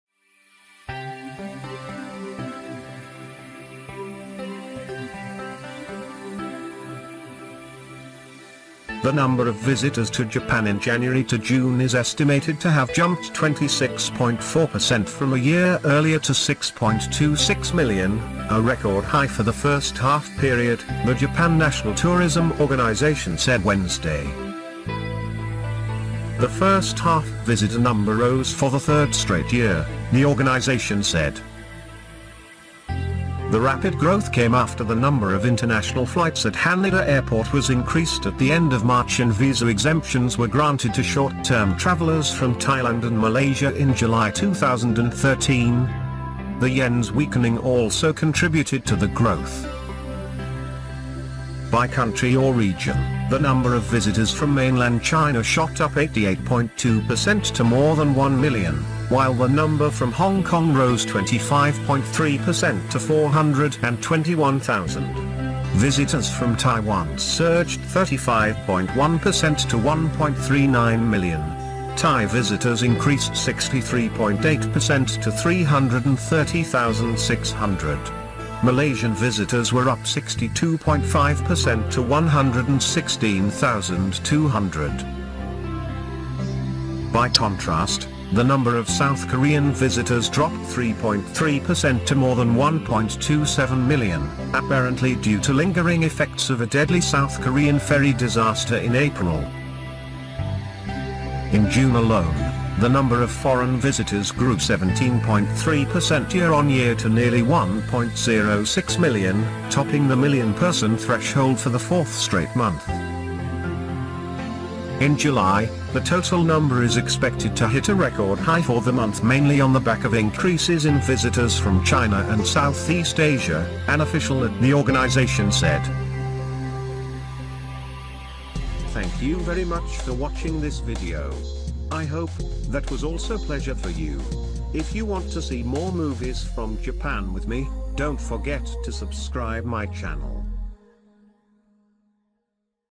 200. Record 6.26 mil. foreign visitors arrive in Jan.-June
The number of visitors to Japan in January-June is estimated to have jumped 26.4 percent from a year earlier to 6.26 million, a record high for the first-half period, the Japan National Tourism Organization said Wednesday. The first-half visitor number rose for the third straight year, the organization said. The rapid growth came after the number of international flights at Haneda Airport was increased at the end of March and visa exemptions were granted to short-term travelers from Thailand and Malaysia in July 2013. The yen's weakening also contributed to the growth. By country or region, the number of visitors from mainland China shot up 88.2 percent to more than 1 million, while the number from Hong Kong rose 25.3 percent to 421,000. Visitors from Taiwan surged 35.1 percent to 1.39 million. Thai visitors increased 63.8 percent to 330,600. Malaysian visitors were up 62.5 percent to 116,200. By contrast, the number of South Korean visitors dropped 3.3 percent to more than 1.27 million, apparently due to lingering effects of a deadly South Korean ferry disaster in April. In June alone, the number of foreign visitors grew 17.3 percent year on year to nearly 1.06 million, topping the million-person threshold for the fourth straight month. In July, the total number is expected to hit a record high for the month mainly on the back of increases in visitors from China and Southeast Asia, an official at the organization said.

Recorded using camera: JVC KENWOOD EVERIO GZ-E765-W